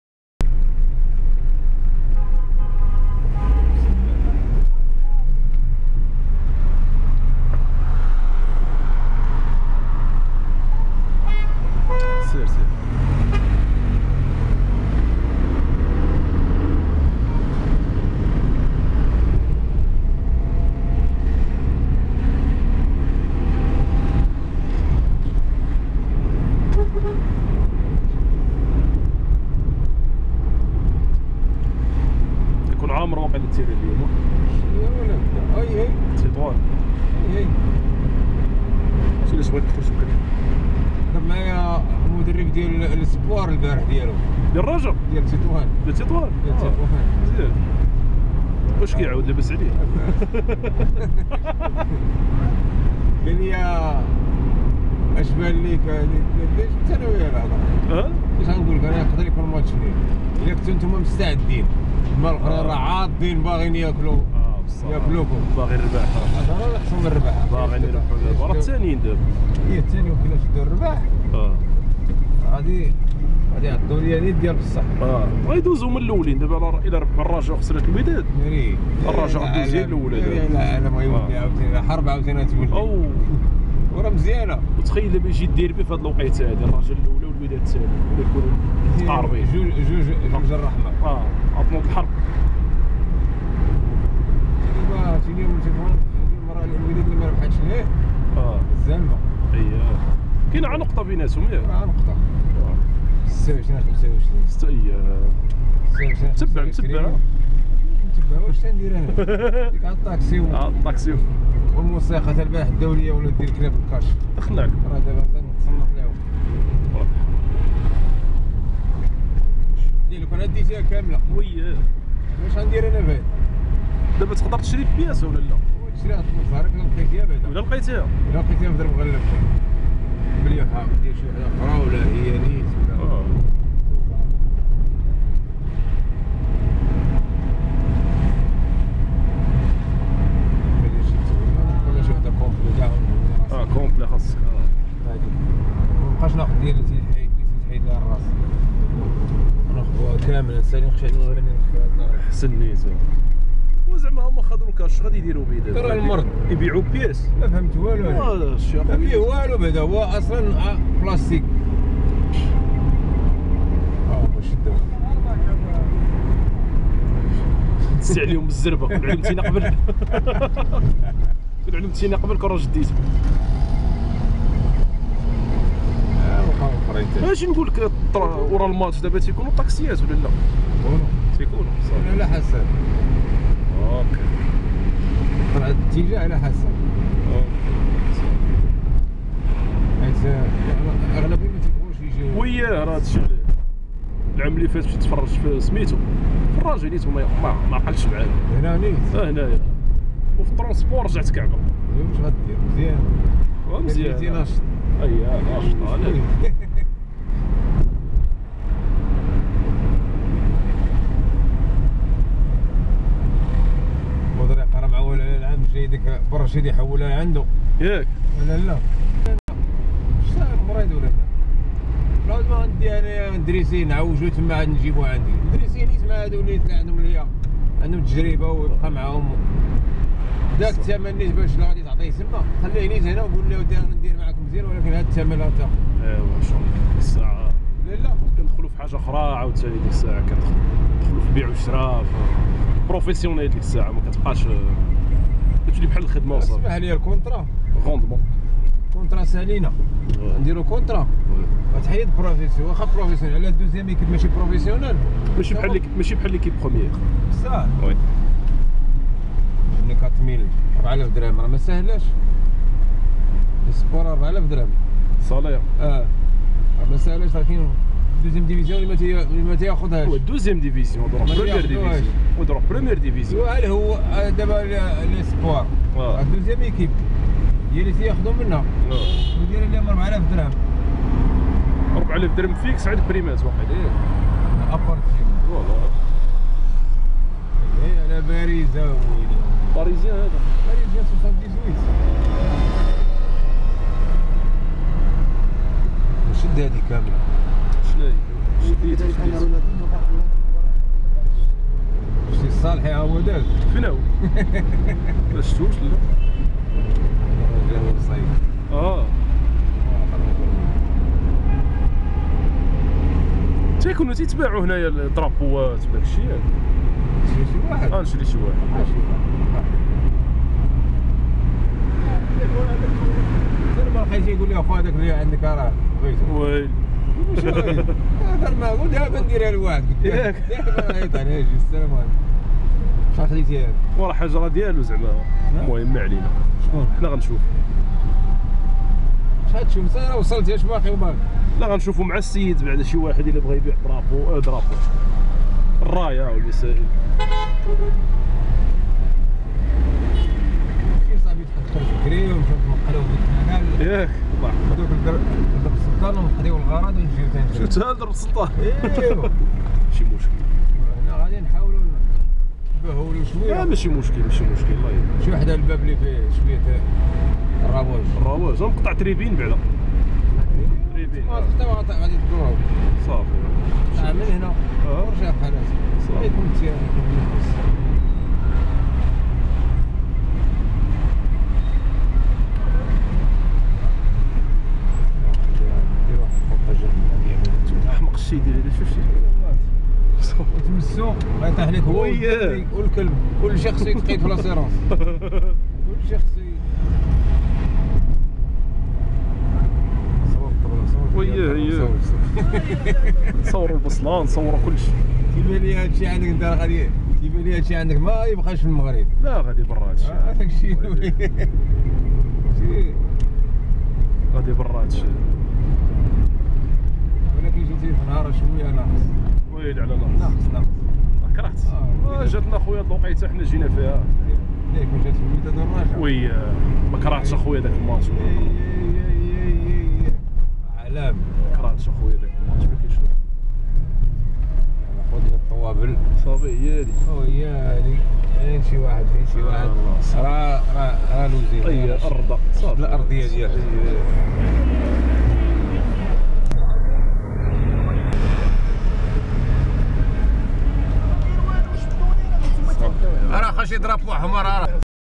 Balade Casablanca, de Bourgogne direction stade Mohamed 5